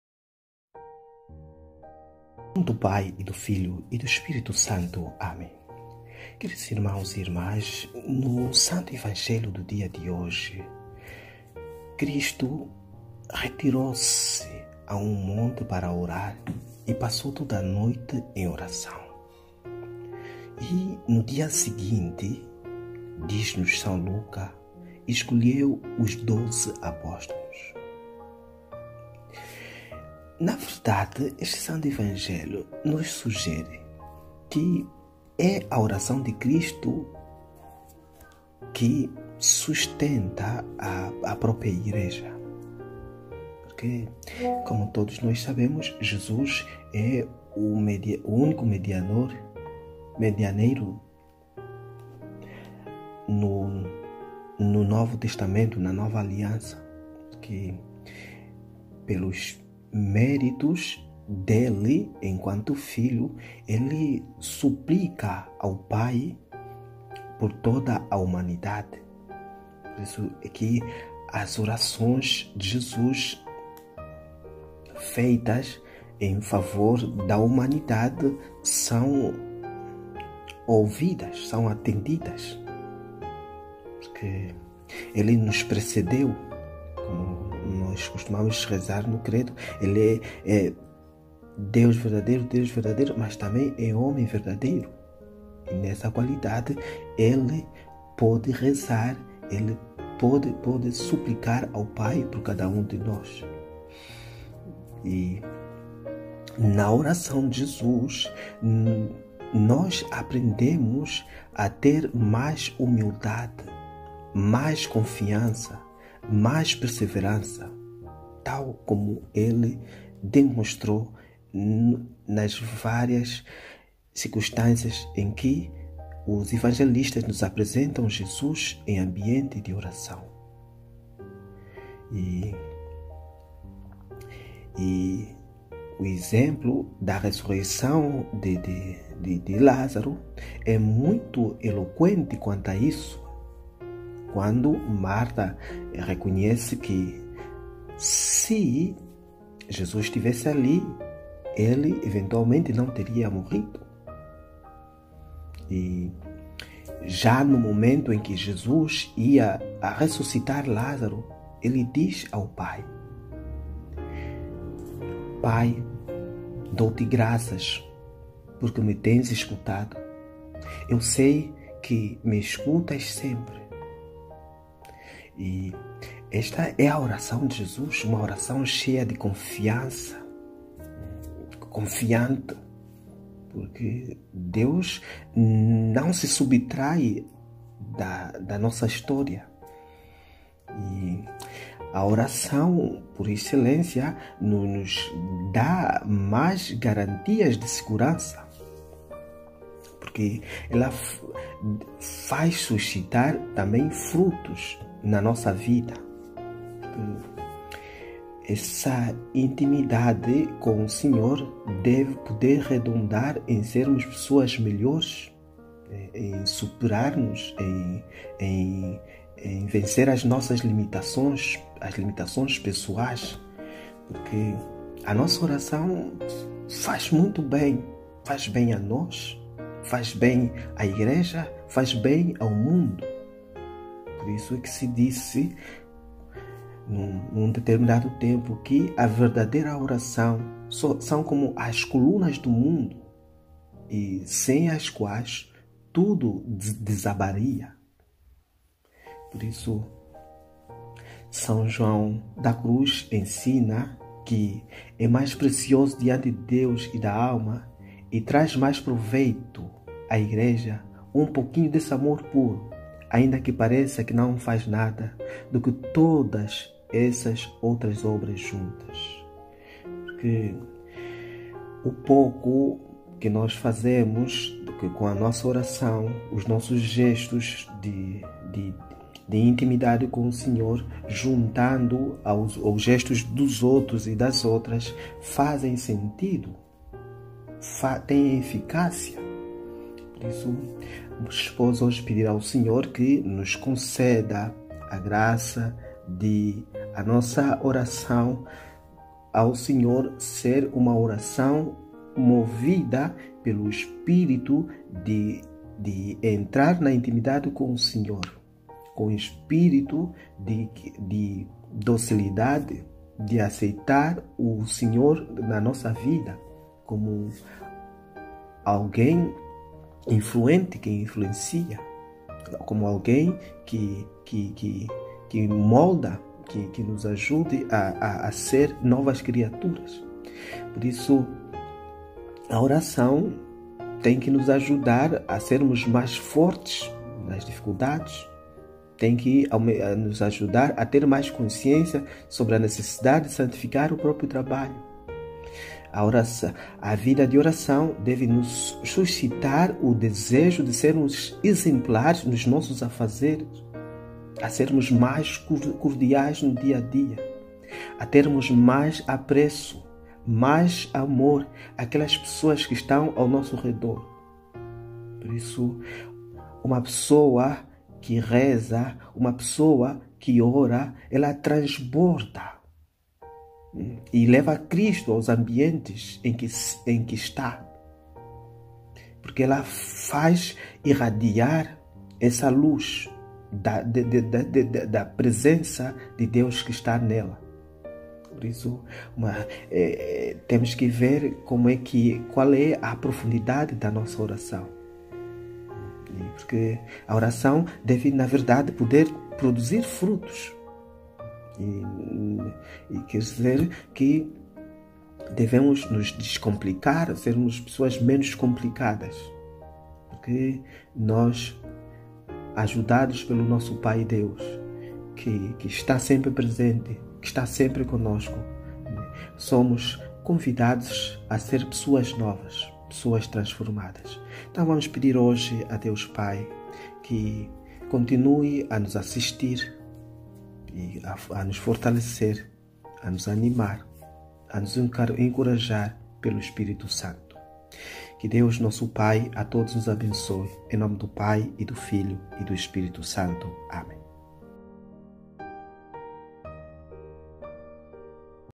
Passou a noite inteira orando e escolheu Doze aos quais também deu o nome de apóstolos
MEDITAÇÃO DA LITURGIA DIÁRIA
Terça-feira XXIII Semana do Comum